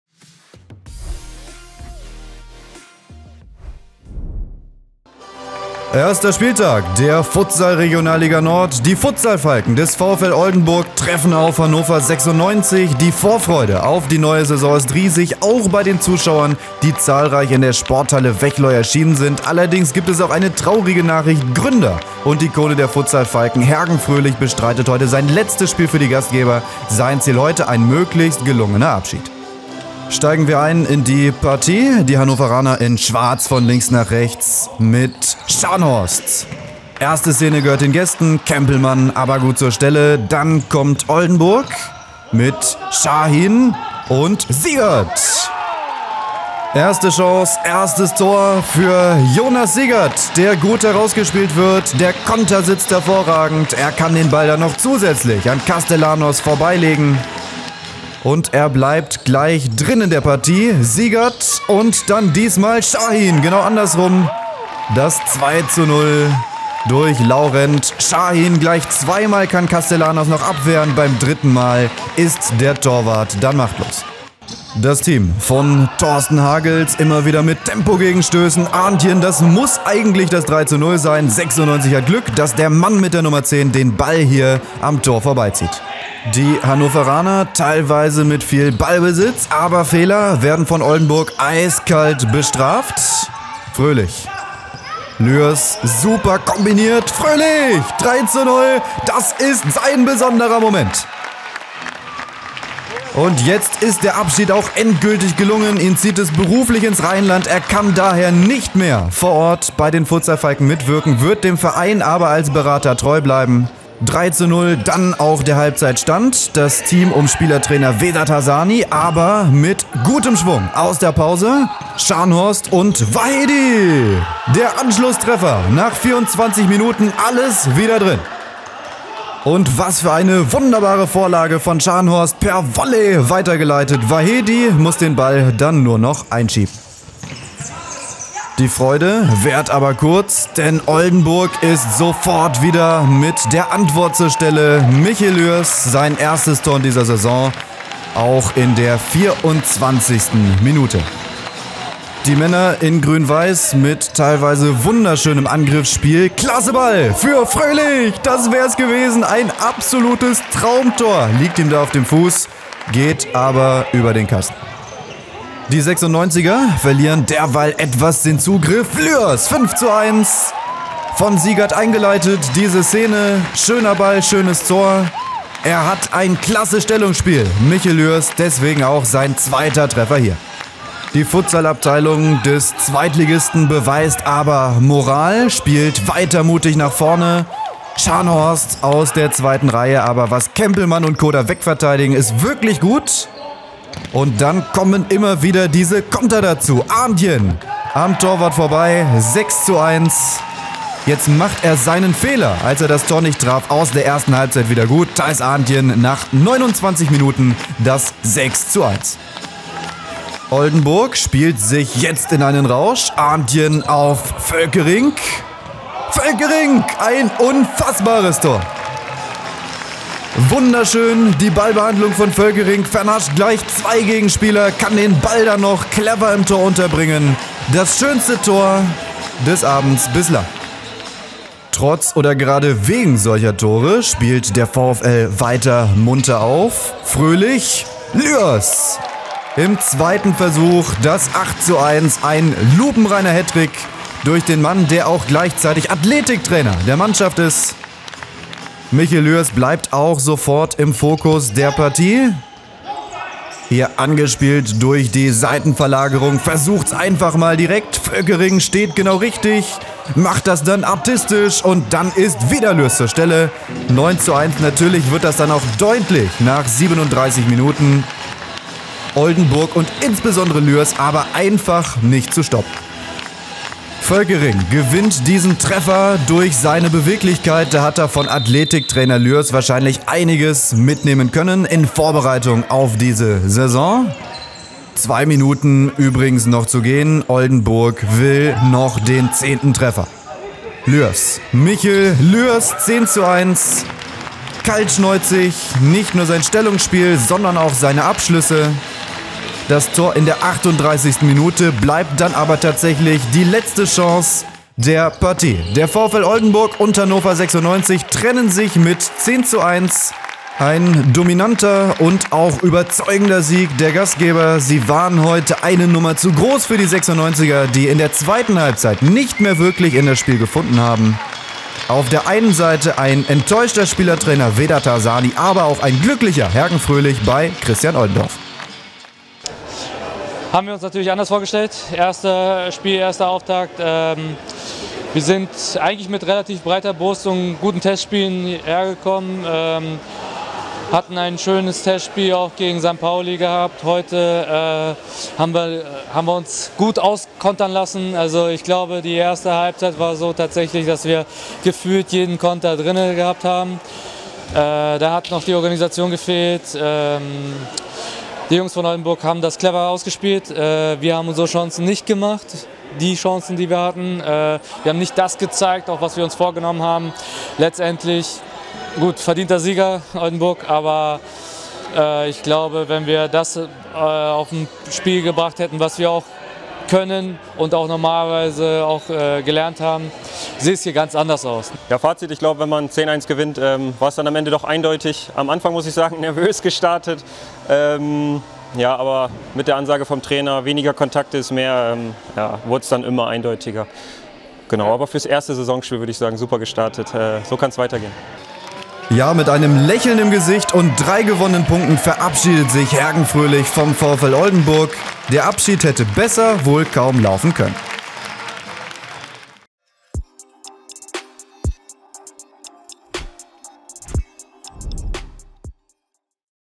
Abschied für VfL Ikone | VfL Oldenburg Futsalfalken – Hannover 96 (Futsal-Regionalliga, 1. Spieltag)
1. Spieltag (18.09.21)

Ergebnis: 10:1 (3:0)
Tore:
1:0 Jonas Siegert
2:0 Laurent Sahin
3:0 Hergen Fröhlich
3:1 Massud Wahedy
4:1 Michel Lüers
5:1 Michel Lüers
6:1 Theis Arntjen
7:1 Joshua Völkerink
8:1 Michel Lüers
9:1 Michel Lüers
10:1 Michel Lüers

Kader VfL Oldenbourg:
1 Daniel Kempelmann, 12 Jasper Hesse, 5 Hergen Fröhlich, 9 Jonas Siegert, 10 Theis Arntjen, 13 Raphael Dourado Goncalves, 14 Joshua Völkerink, 15 Michel Lüers, 20 Thorsten Hagels, 22 Daniel Schäfer, 33 Laurent Sahin

Trainer: Thorsten Hagels

Kader Hannover 96:
1 Samet Sahin, 22 Ismal Perez Castellanos, 3 Massud Wahedy, 5 Patrick Koch, 6 Sarmed Ahmed, 8 Jonas Maulud, 9 Verdat Hasani, 15 Lah Smajli, 17 Christian Scharnhorst, 29 Andres Garaves Baron

Trainer: Vedat Hasani

Kommentar: Jan Ole Behrens
Produktion: Christian Oldendorf